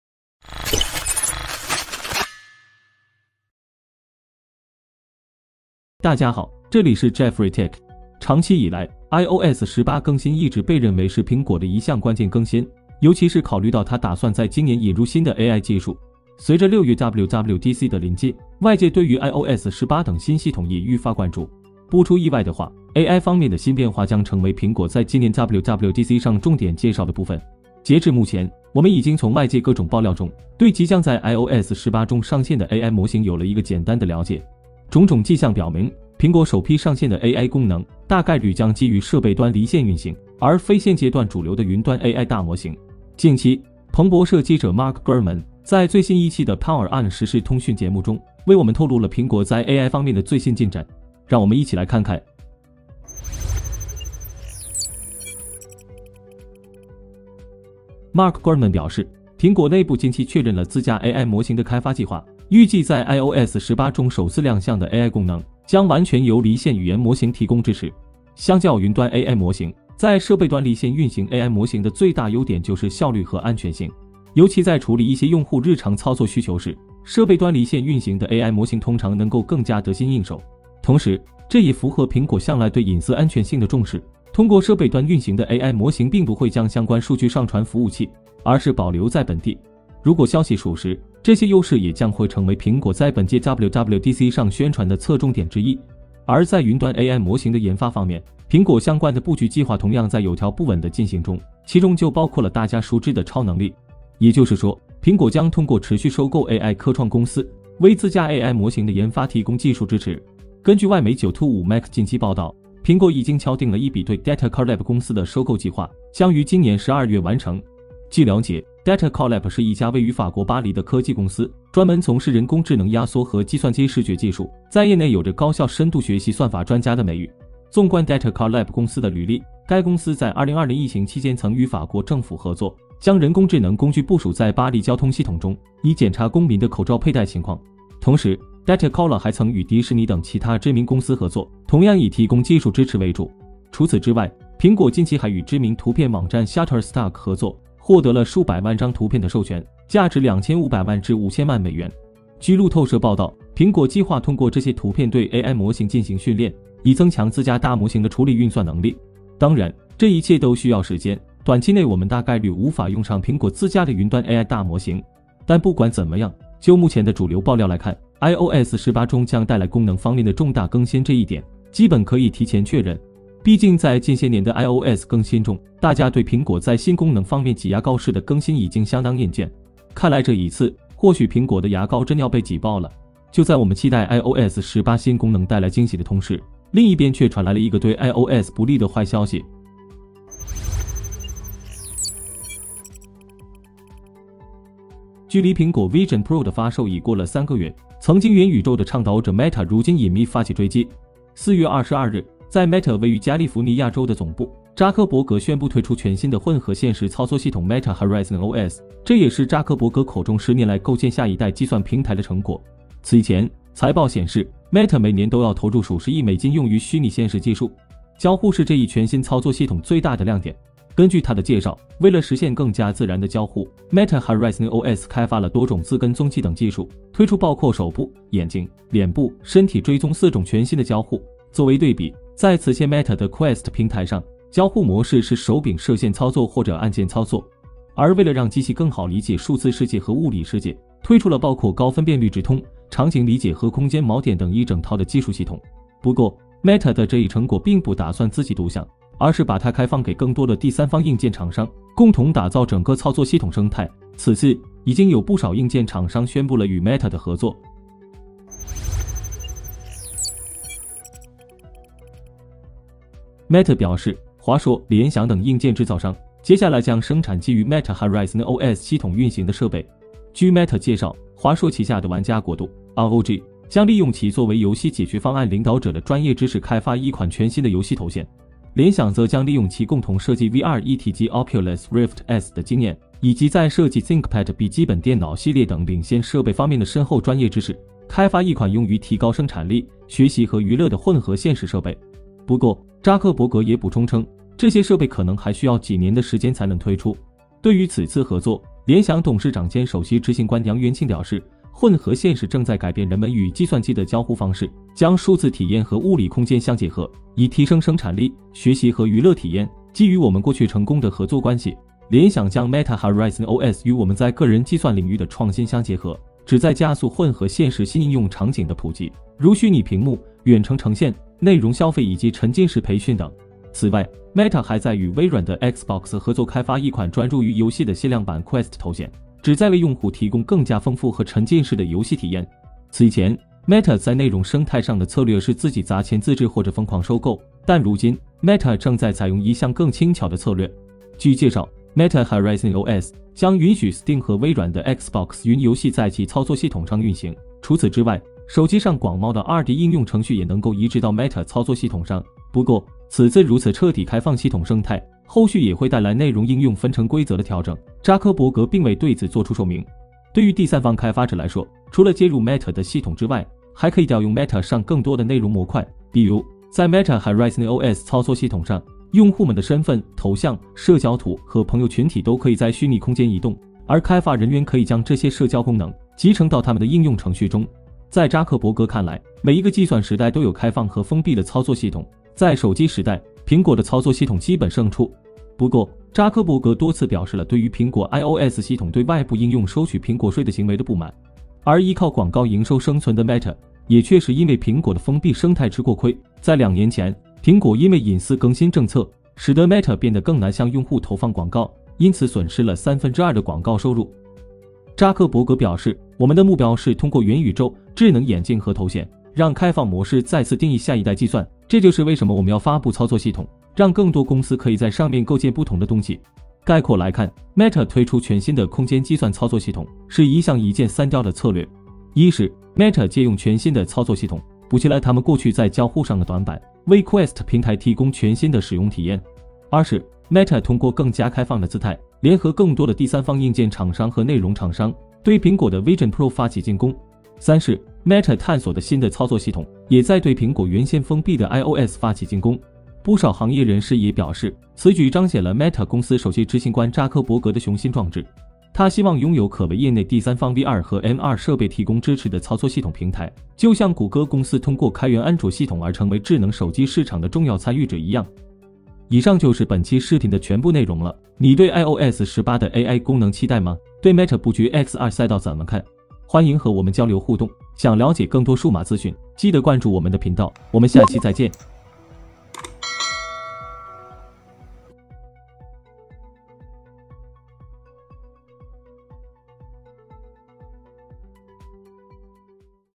苹果iOS 18 AI模型最新进展！扎克伯格开放Meta Horizon OS头显操作系统，向iOS发起冲击，联想华硕已“入伙”，MR行业要变天？【JeffreyTech】
截至目前，我们已经从外界各种爆料中，对即将在 iOS 18 中上线的 AI 模型有了一个简单的了解。
种种迹象表明，苹果首批上线的 AI 功能，大概率将基于设备端离线运行，而非现阶段主流的云端 AI 大模型。
近期，彭博社记者 Mark Gurman 在最新一期的 Power On 时事通讯节目中，为我们透露了苹果在 AI 方面的最新进展。
让我们一起来看看。……

00:00 iOS 18牙膏挤爆
00:53 iOS 18推出首批全新AI功能
03:10 Meta推出空间计算操作系统 对iOS发起冲击
04:18 华硕联想等已“入伙”